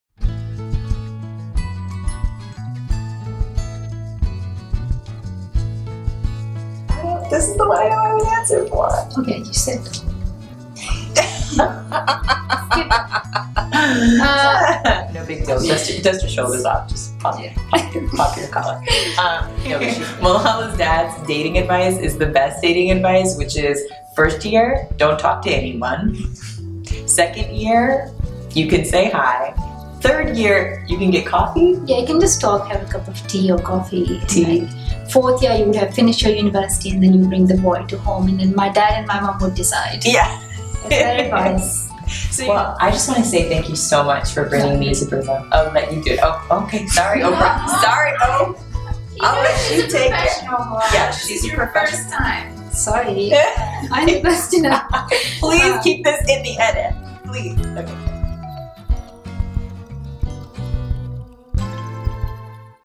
Bloopers, dating advice and Malala's attempt at being Oprah
Here are the funniest moments from Malala's chat with her first ever interviewee Elaine Welteroth, a renowned journalist and former Editor-in-Chief of Teen Vogue.